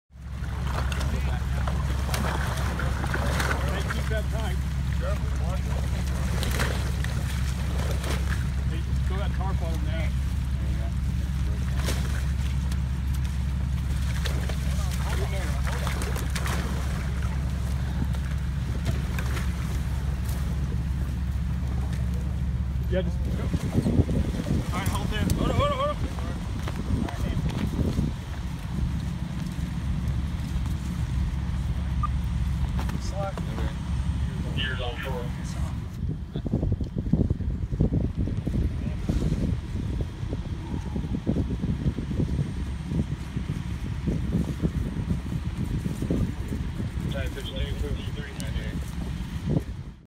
Buz tutmuş gölde sıkışan geyik için kurtarma operasyonu
Amerika Birleşik Devletleri'nde buz tutmuş gölde mahzur kalan geyik itfaiye görevlileri tarafından kurtarıldı. …